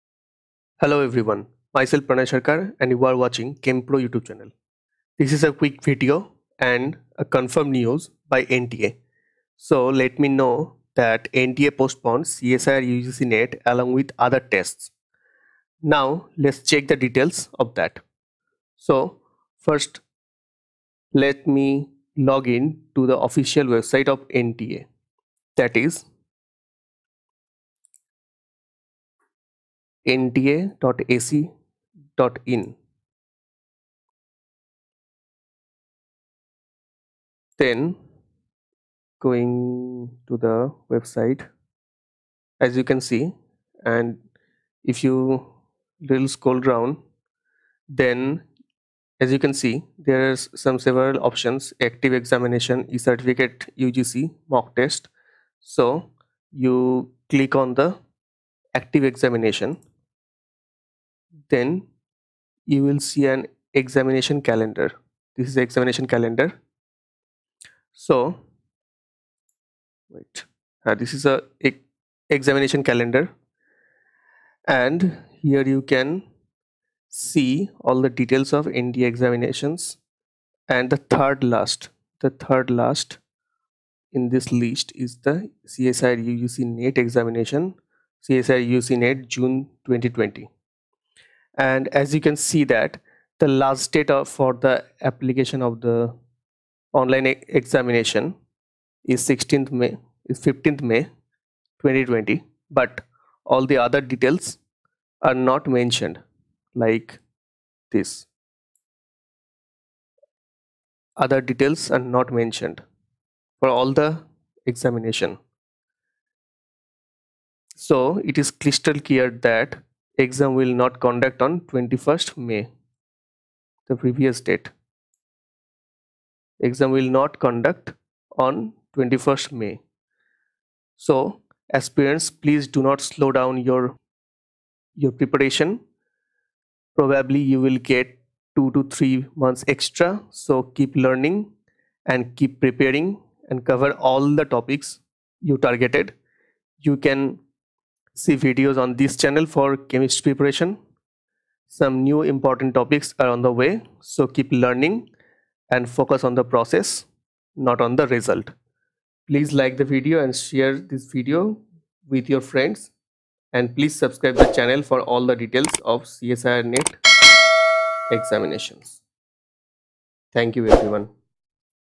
NTA POSTPONED CSIR UGC NET JUNE 2020 I UGC NET EXAM POSTPONED I NEW DATE OF CSIR UGC NET
NTA POSTPONED CSIR UGC NET JUNE 2020 along with other examinations conducted by NTA. Dates are  to be decided later on. When will csir net be conducted?

" Sorry for the mistake, the previous date for CSIR-UGC NET was 21st June not 21st May"

CHEM PRO
ChemPro